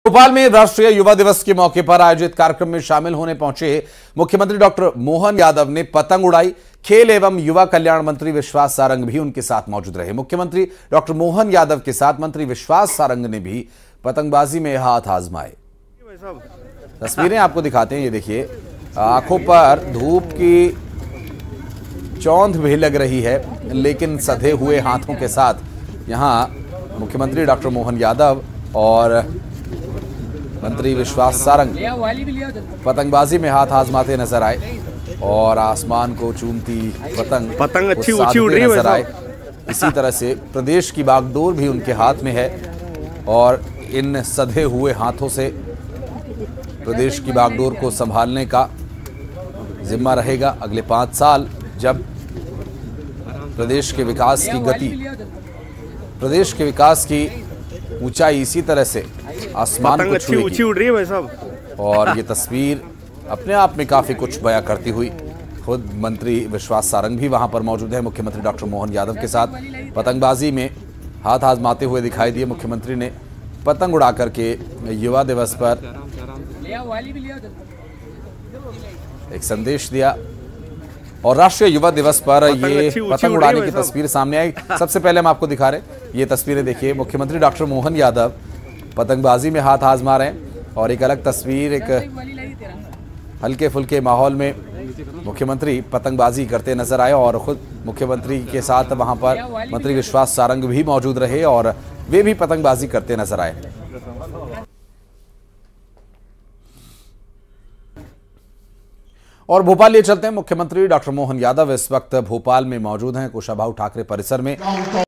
National Youth Day 2024 : राष्ट्रीय युवा दिवस के मौके पर CM Mohan Yadav ने की पतंगबाजी | MP News
National Youth Day 2024 : राष्ट्रीय युवा दिवस के मौके पर Bhopal में CM Mohan Yadav ने पतंग उड़ाई. खेल एवं युवा कल्याण मंत्री Vishwas Sarang भी उनके साथ मौजूद रहे.